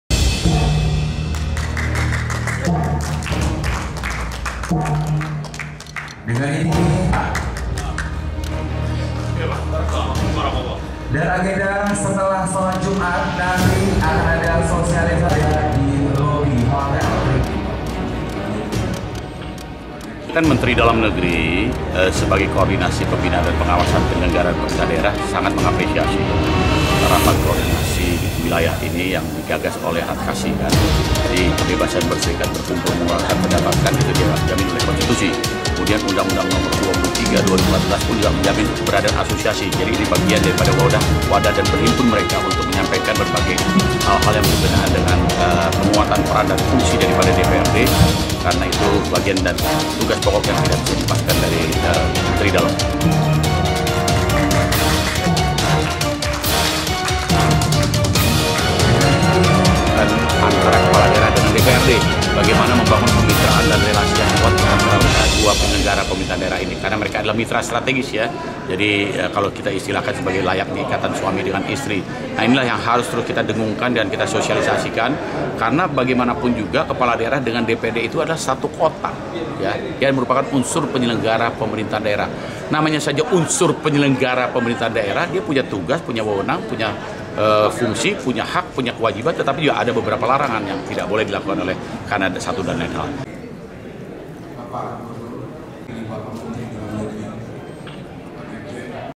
DPRD se Indonesia Gelar Rakorwil di Batam
Laporan Reporter Tribunnews Video, Hadi Maulana

BATAM.TRIBUNNEWS.COM, BATAM – Rapat Koordinasi Wilayah (Rakorwil) asosiasi DPRD kabupaten seluruh indonesia (adkasi) di Batam terlihat sepi.

Bahkan dari se_indonesia yang hanya DPRD se-Prov Nangroe Aceh Darusalam, Sumut, Sumbar, Riau, Jambi, Sumsel, Bengkulu, Bangka Belitung, Lampung dan Kepri sendiri.

Dimana koorwil ini merupakan korwil adkasi yang tiga dan dilaksanakan di Batam Kepulauan Riau.

Dirjen Bina Keuangan Daerah Kemendagri. Reydonnyzar Moenek mengaku kegiatan ini mendapatkan apresiasi dari Mendagri.

“Ini bagian dari wadah asosiasi untuk menghimpun hal-hal yang sesuai dengan peran DPRD kabupaten se-Indonesia sendiri.

Selian itu, nantinya dari korwil ini akan merekomendasikan rumusan kebijakan peran serta merek dalam kedudukan daerah.